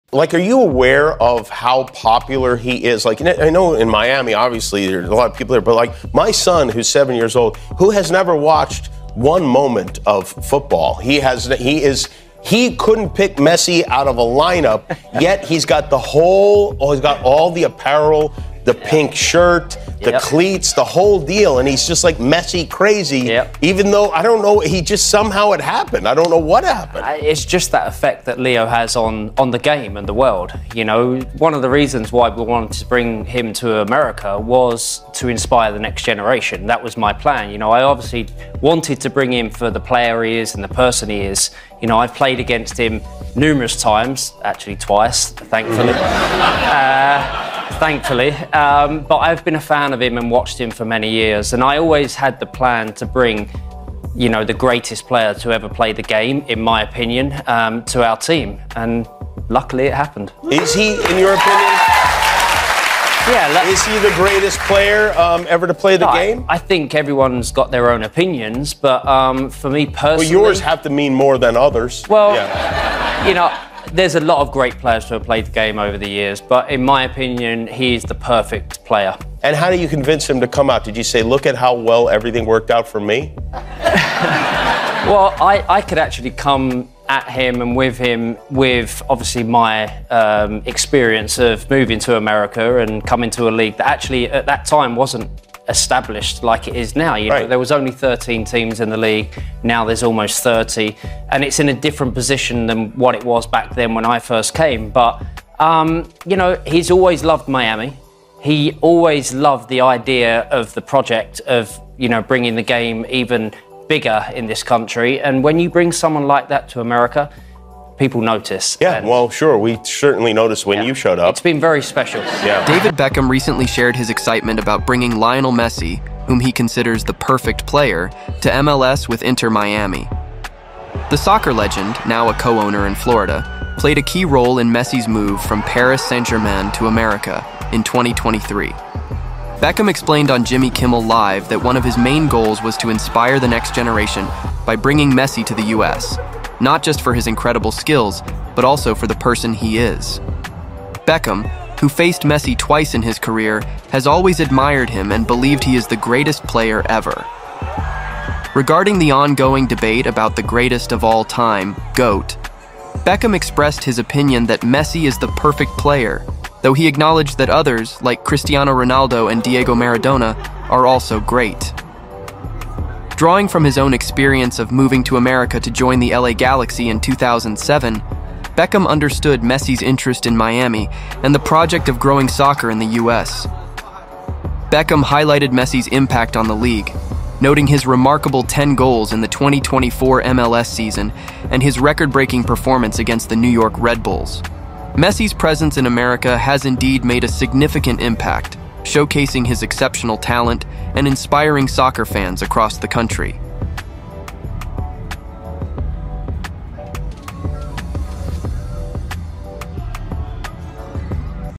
Why exceptional player’ Lionel Messi was ‘always part of David Beckham transfer plan at Inter Miami
Lionel Messi Inter Miami MLS